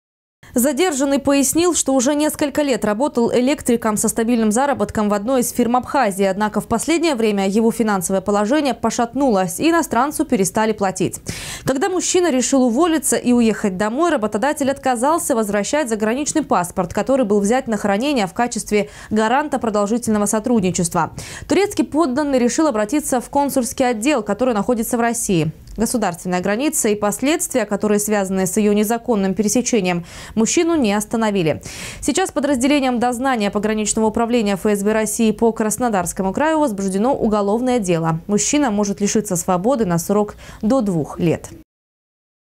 В Сочи задержан нарушитель границы. Новости 24 Сочи Эфкате
Гражданина Турции, переплывшего реку Псоу, задержали сочинские пограничники.44-летний мужчина рассказал, что перейти российско-абхазскую границу в обход пункта пропуска заставили обстоятельства – работодатель не вернул ему паспорт, когда тот захотел вернуться домой.
Сочи. Новости.
Телеканал Эфкате.